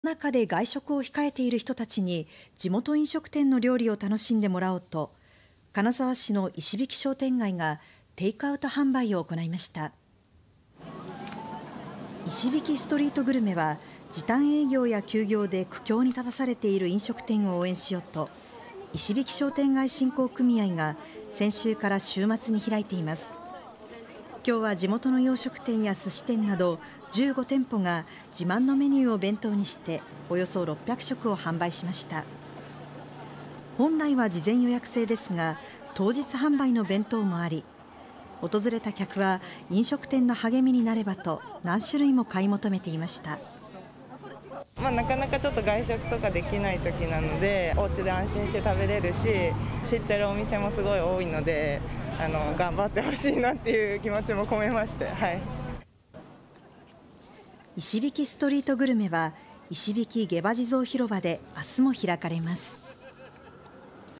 金沢の商店街が地元飲食店を応援 2021.6.12放送
コロナ禍で外食を控えている人たちに、地元飲食店の料理を楽しんでもらおうと、金沢市の石引商店街が１２日、テイクアウト販売を行いました。「石引ストリートグルメ」は、時短営業や休業で苦境に立たされている飲食店を応援しようと、石引商店街振興組合が先週から週末に開いています。１２日は地元の洋食店や寿司店など１５店舗が、自慢のメニューを弁当にして、およそ６００食を販売しました。本来は事前予約制ですが、当日販売の弁当もあり、訪れた客は、「飲食店の励みになれば」と、何種類も買い求めていました。石引ストリートグルメは石引下馬地蔵広場で、１３日も開かれます。

月～金　18:15~

11:41~　／　20:54~